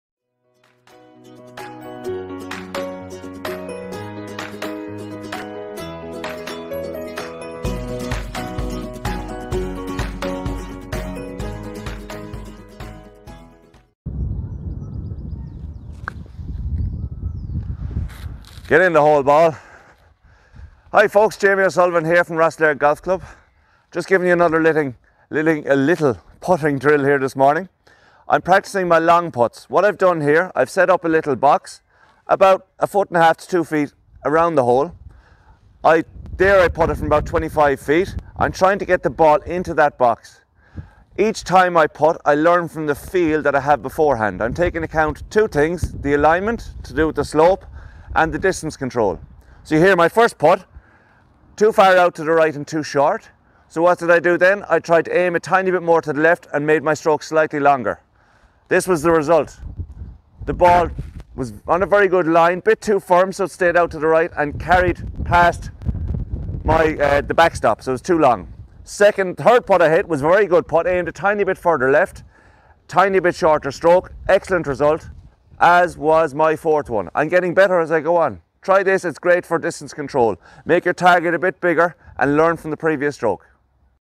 Thursday Tips with Jamie O'Sullivan - Putting Drill: Learn from previous stroke
Today Jamie chats us through a drill that will help with our long putting. It's all about learning from the previous stroke.. it's a simple drill but very effective in helping control your distance with those tricky long putts.